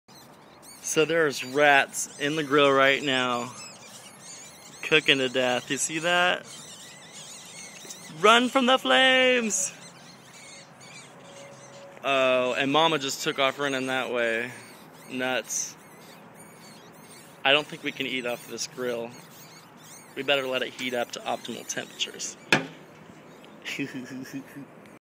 Grill ratz
Baby rats that nested in my grill.. Who turned on the grill? WTF?